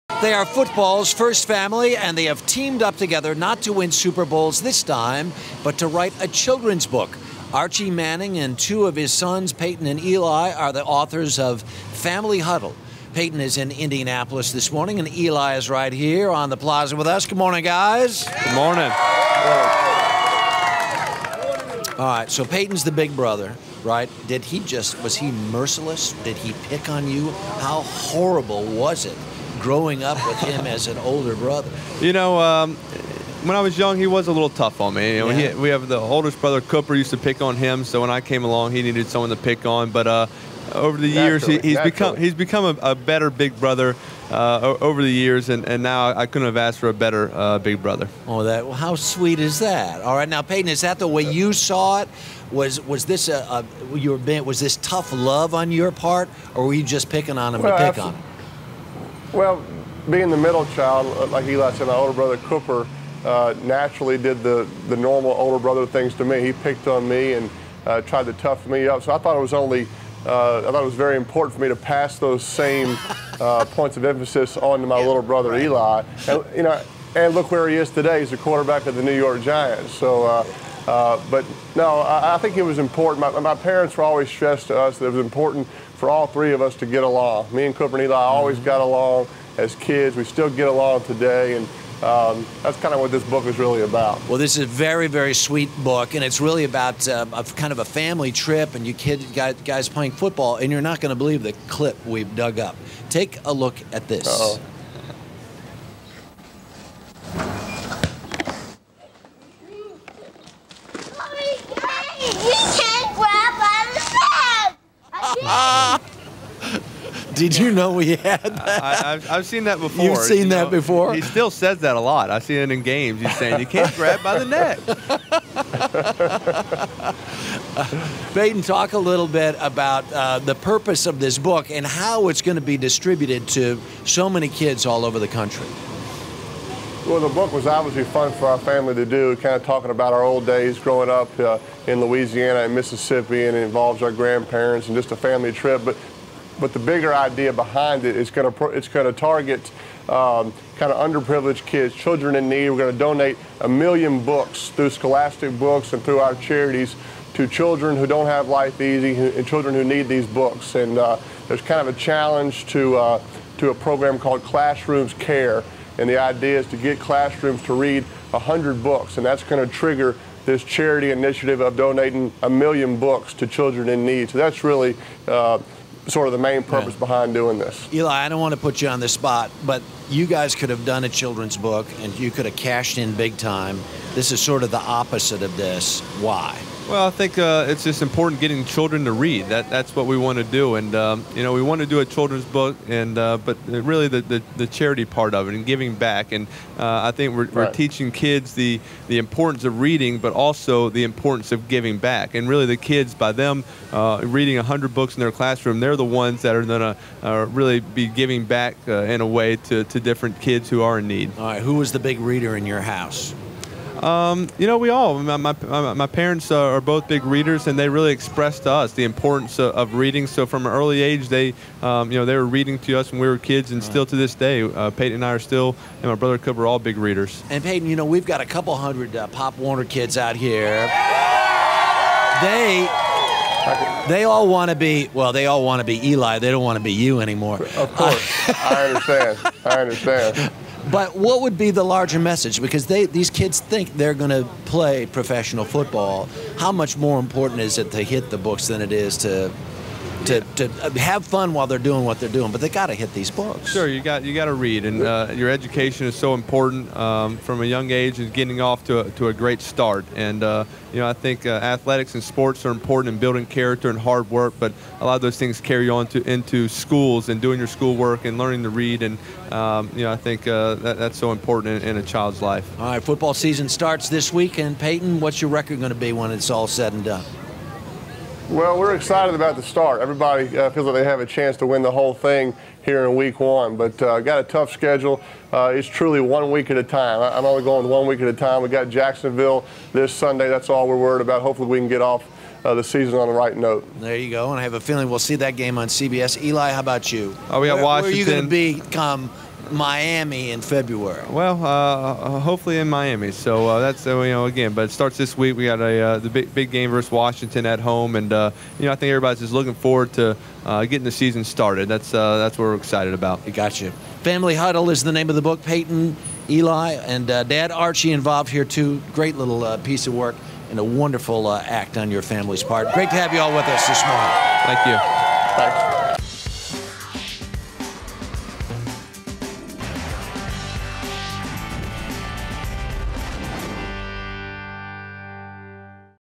Manning Brothers' New Book
NFL stars Peyton and Eli Manning spoke with Harry Smith about their new children's book, Family Huddle.